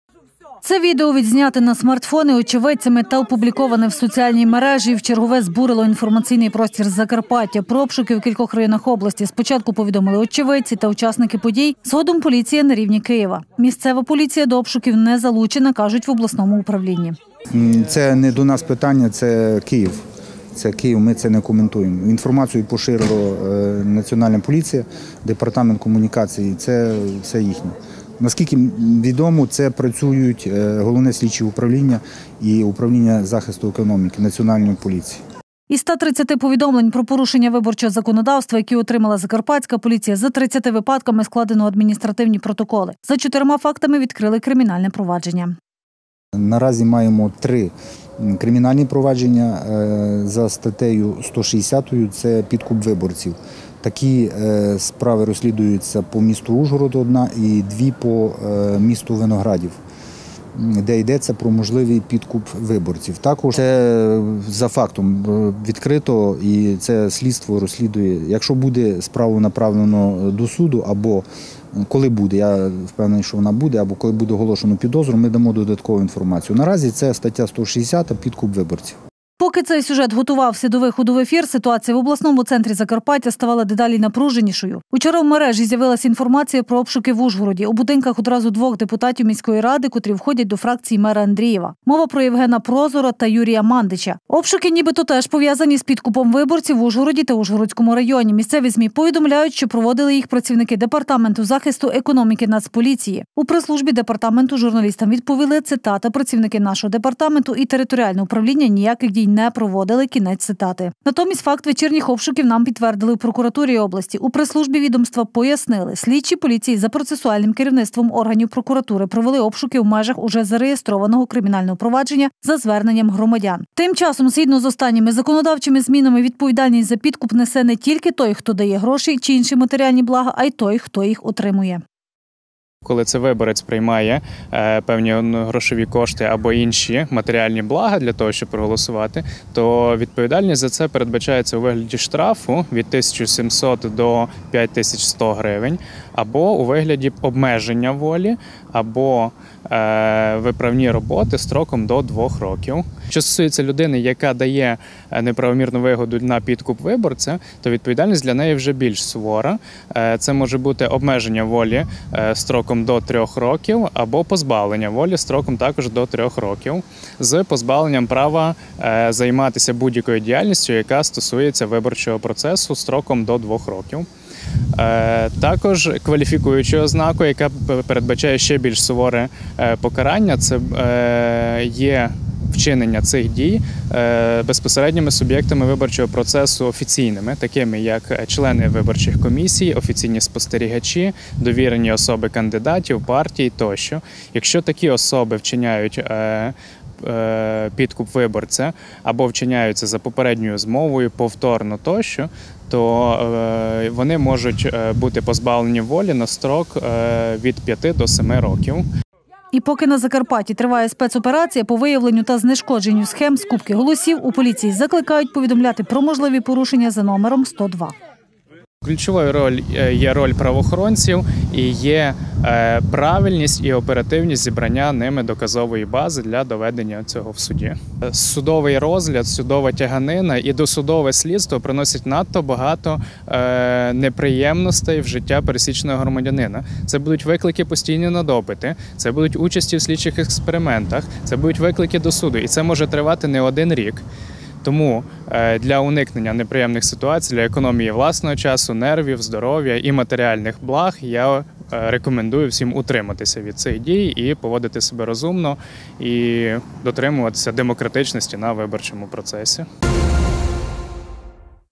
Підкуп виборців
Майже півтори сотні повідомлень про порушення виборчого законодавства і три кримінальних провадження за фактом підкупу виборців. Таку статистику за чотири дні до дня голосування фіксують закарпатські правоохоронці. За неофіційною інформацією, обшуки у справі підкупу виборців вже почалися у домівках депутатів Ужгородської міської ради. Скільки ж загрожує вам, якщо ви даватимете чи братимете матеріальну винагороду за голосування? У сюжеті розповідаємо, як на Закарпатті розкривають схему скупки голосів на парламентських виборах, та на скільки років можна опинитися за ґратами за продаж вашого голосу.